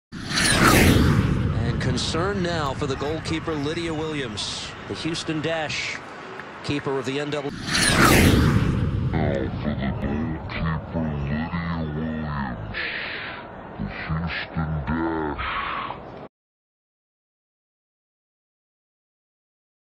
Was poor hydration to blame for the Matilda's loss?
This video shows that a lack of hydration may have been the cause of the Matilda's loss.